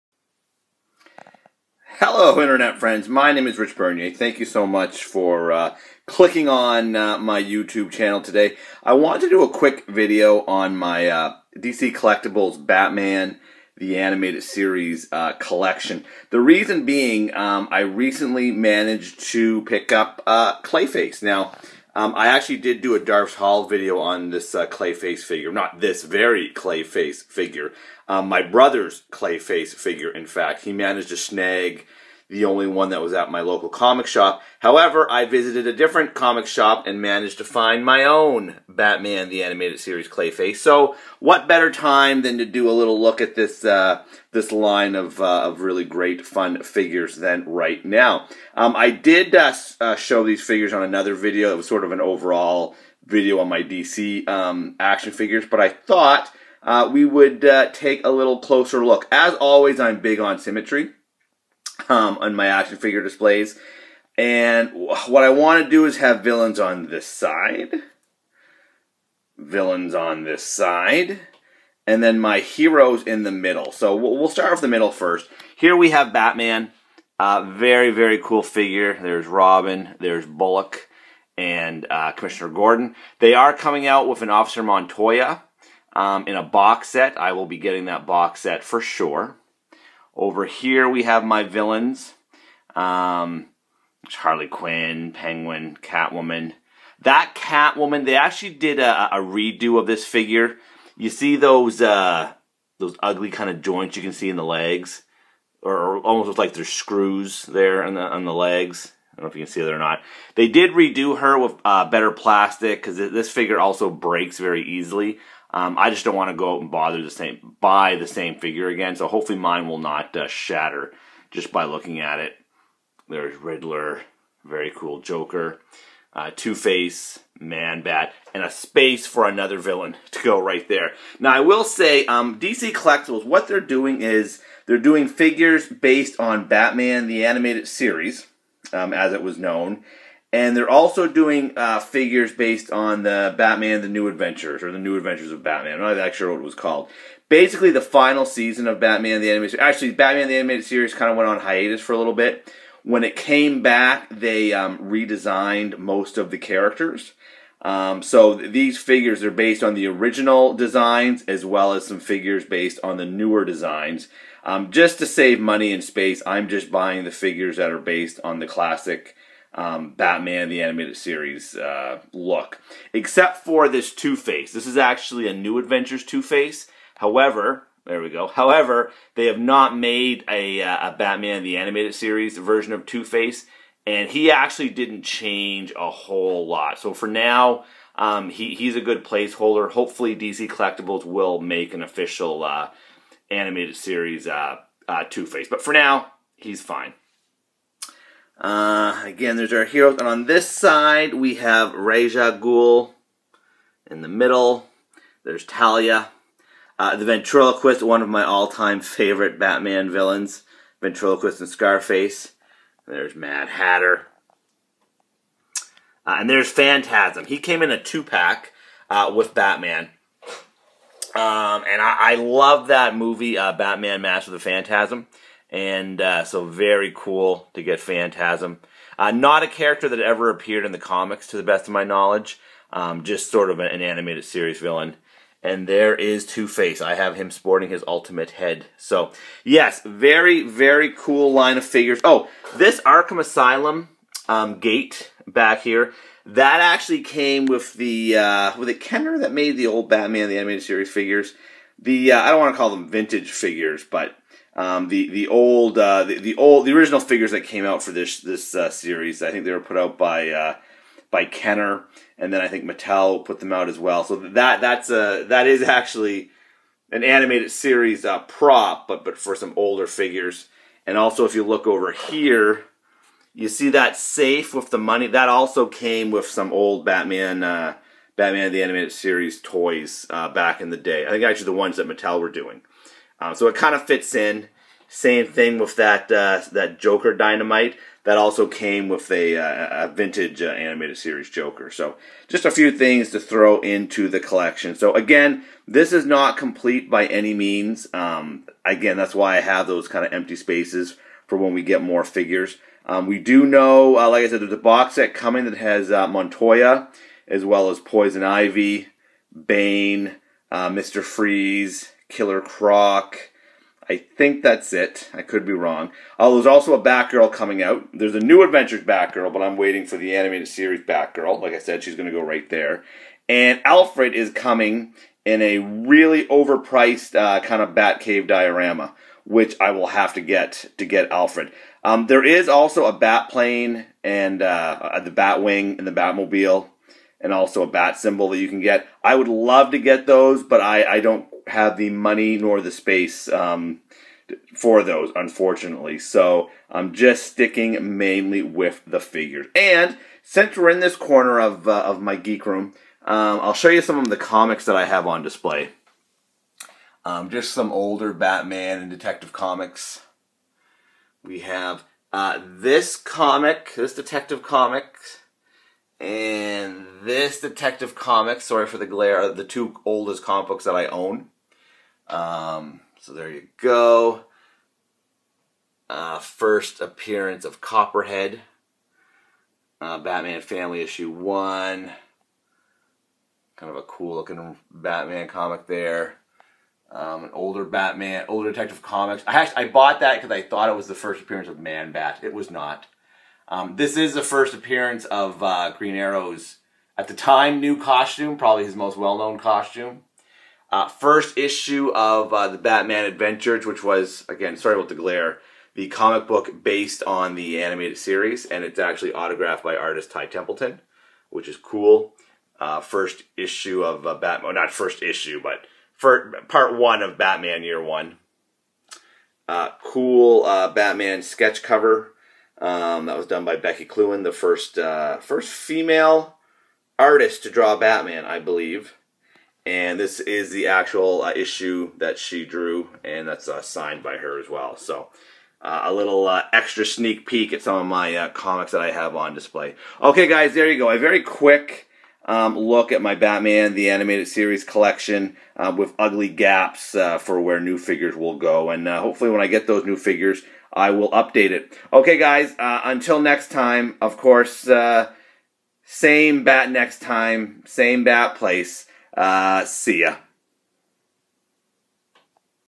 DC Collectibles Batman The Animated Series Action Figure Collection Tour
What kind of a Batman fan would I be if I didn't LOVE Batman: The Animated series? Not a very good one, that's what kind.